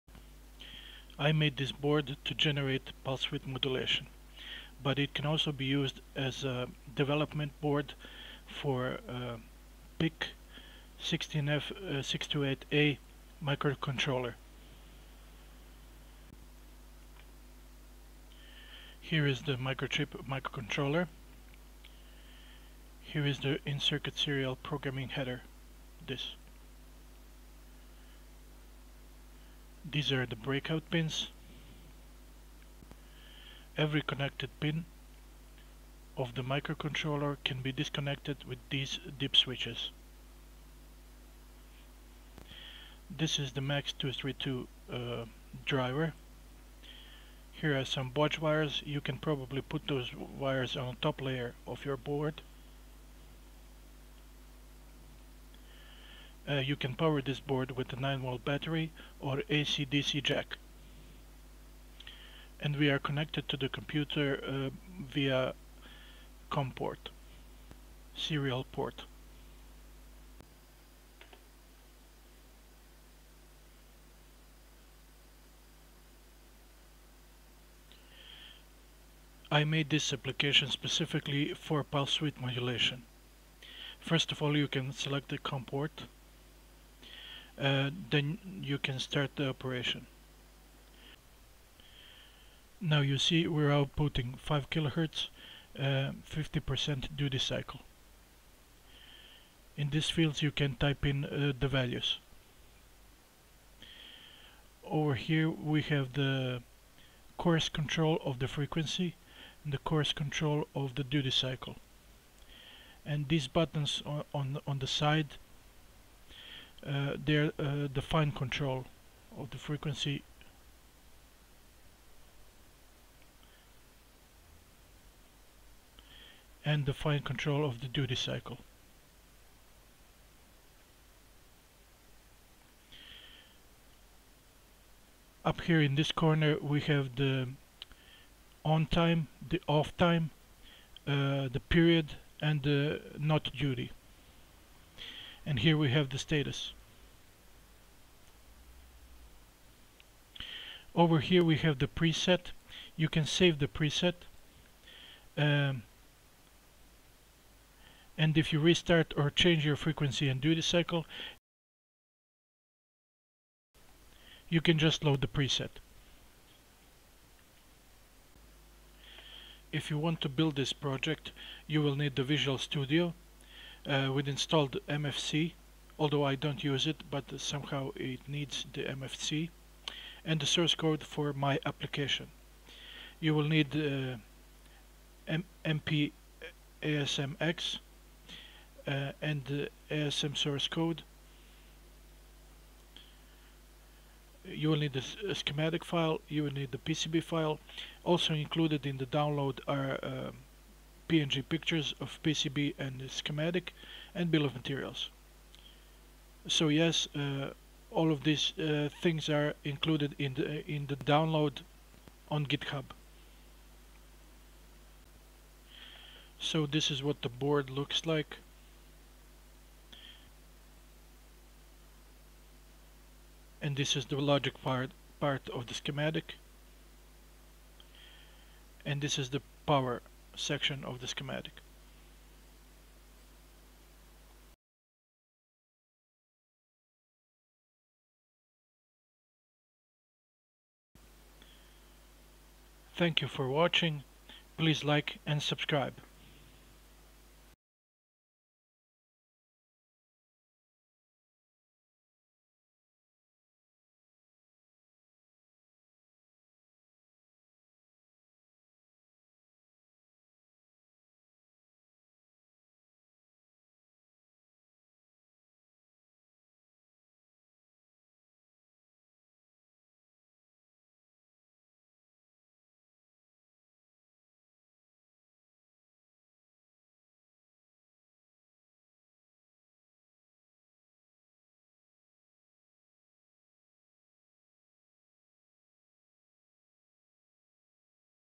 PWM using PIC16F628A and a PC
I made this board to generate pulse width modulation.
But it can also be used as a development board for pic16f628a.

Updated to have command line arguments.
Format:  exe com freq duty
You can put this in a batch file or shortcut.